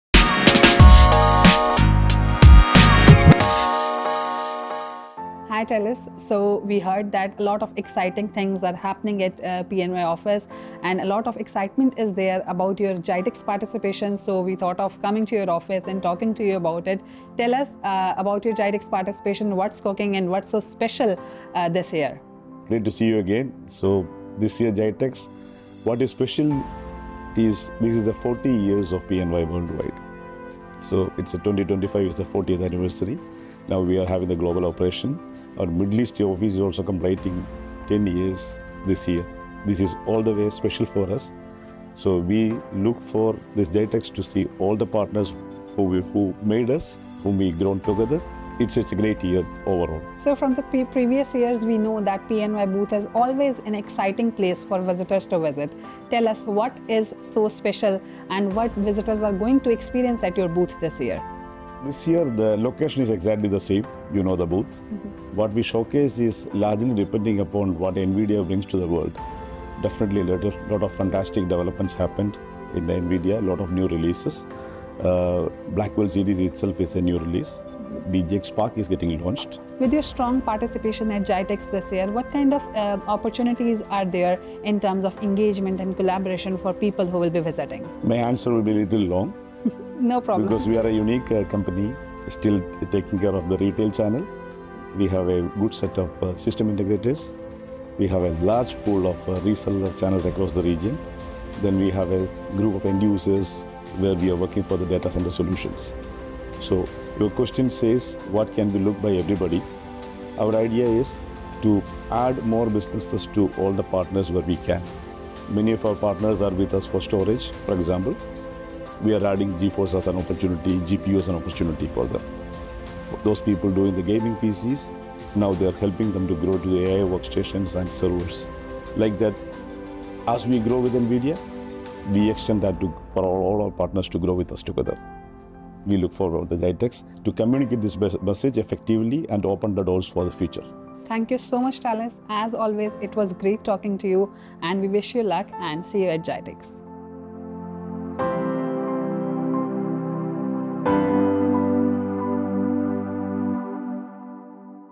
PNY Technologies at GITEX 2025 | 40 Years of Innovation with Talus Arukalil
Join Talus Arukalil, Regional Manager MEAI at PNY Technologies, as he shares the excitement around PNY’s 40th anniversary and 10 years in the Middle East. Discover what’s new at PNY’s GITEX 2025 booth, including the latest Nvidia releases, AI workstations, and opportunities for partners and end users.

Watch now to see how PNY is shaping the future of gaming, AI, and data center solutions across the region!

Key Highlights:
🟦 40th Anniversary of PNY worldwide
🟩 10 Years of Middle East operations
🟨 Nvidia Blackwell Series & BJX Park showcase
🟥 Business growth opportunities for partners & resellers